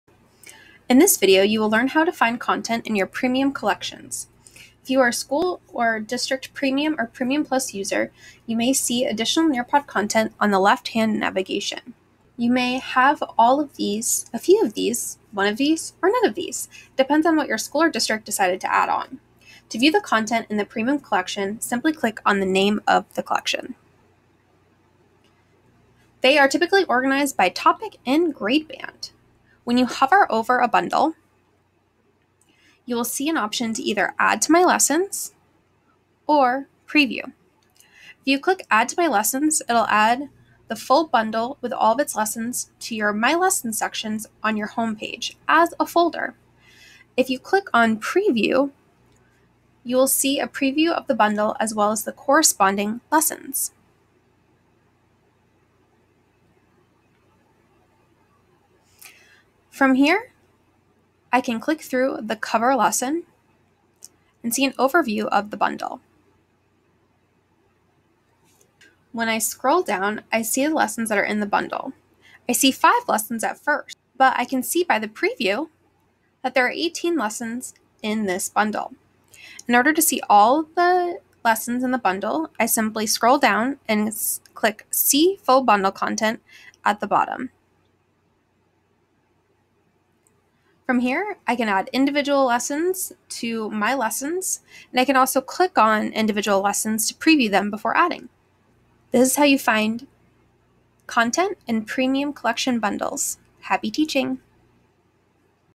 How to find content in Premium Collections bundles
In your Nearpod Premium or Premium Plus license, you may have access to special content in Premium Collections. In this video, you will learn how to browse the collections you have access to and be able to add them to your lessons to use with your students